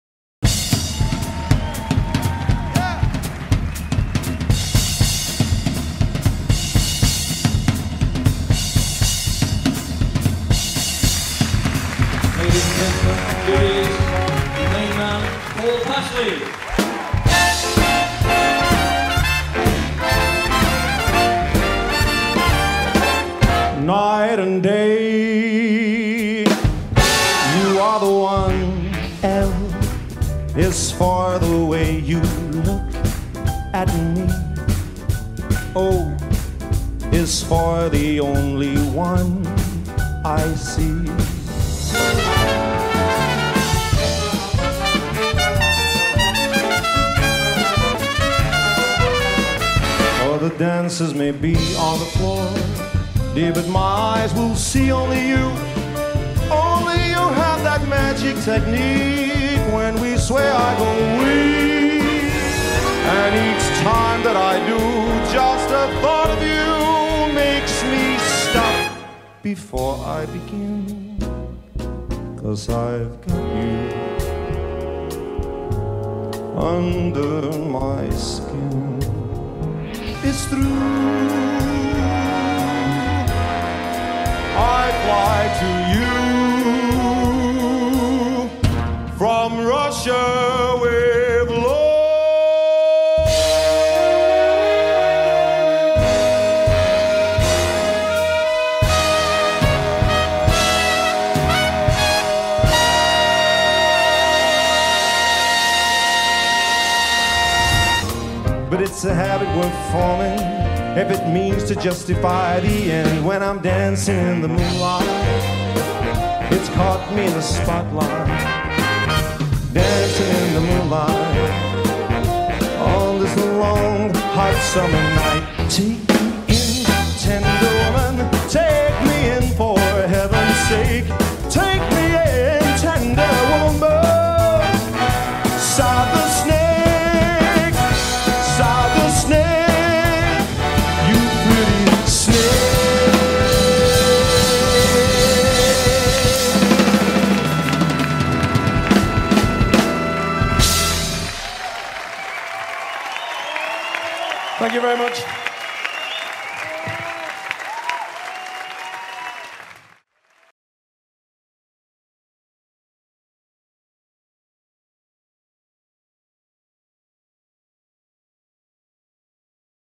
Paul Pashley Promotional Video
A promotional highlights video for Paul Pashley a big band singer, recorded at the Crucible, Sheffield.

To get the ball rolling on your own creative video project, visit us